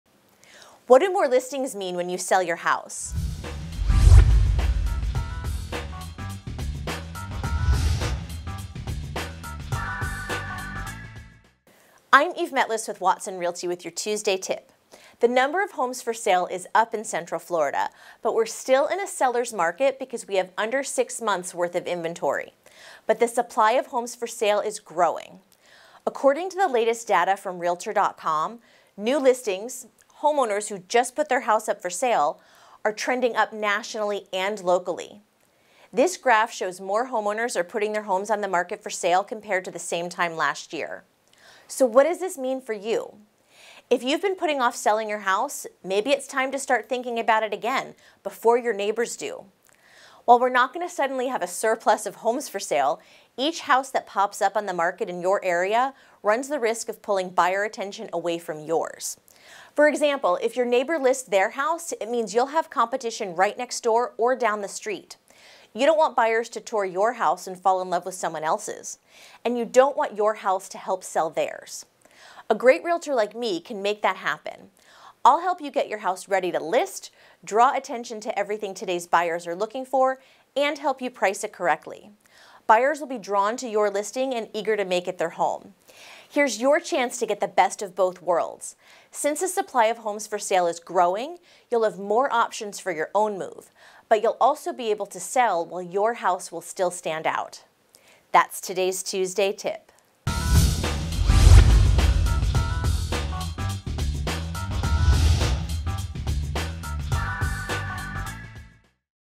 Why More Listings Matter When Selling Your Home!
Learn how the increase in home listings impacts your ability to sell your house in Central Florida. Eve Metlis from Watson Realty shares insights and tips to ensure your home stands out in a growing market. Don't miss this essential advice for sellers!